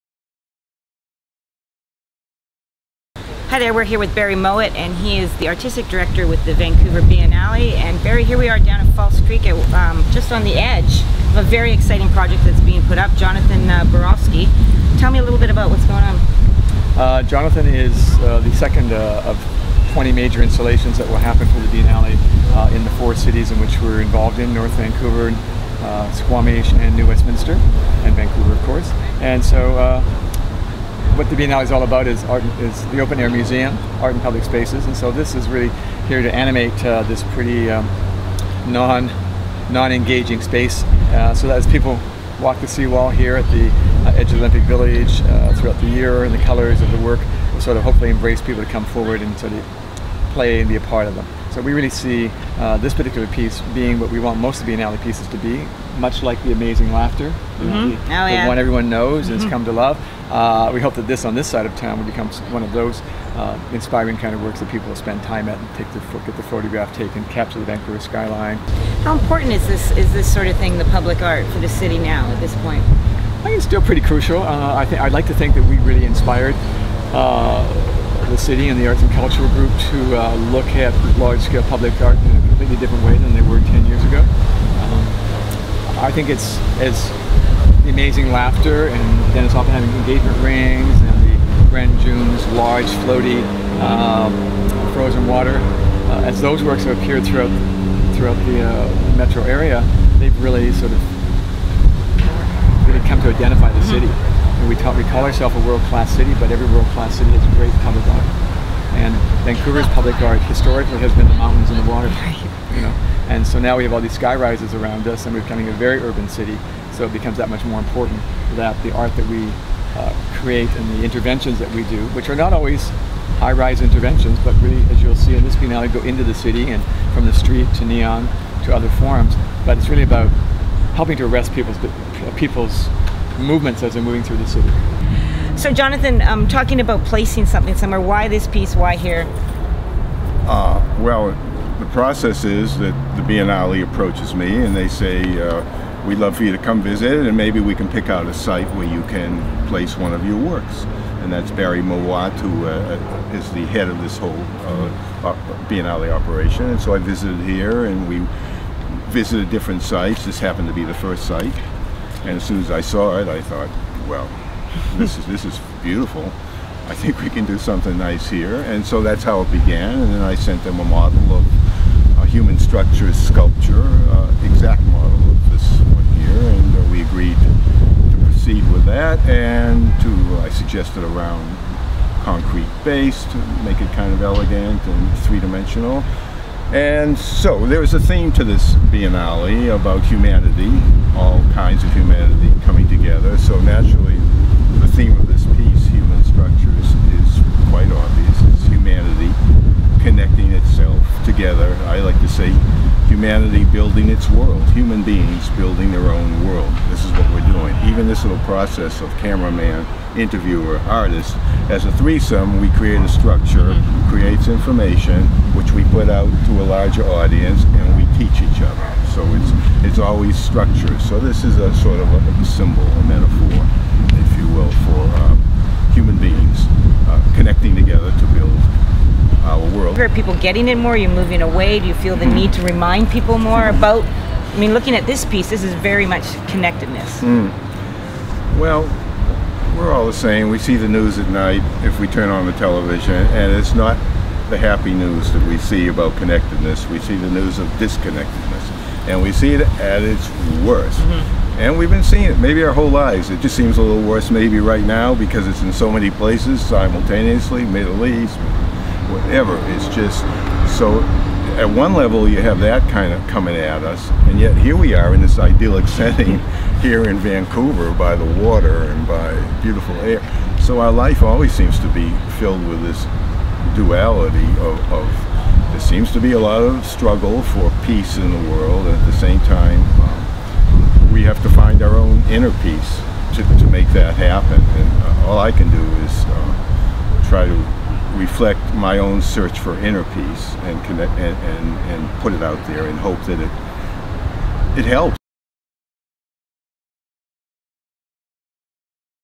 Human Structures in False Creek shows connectedness between people, artist says
Sculptor Jonathan Borofsky talks about his piece Human Structures on False Creek.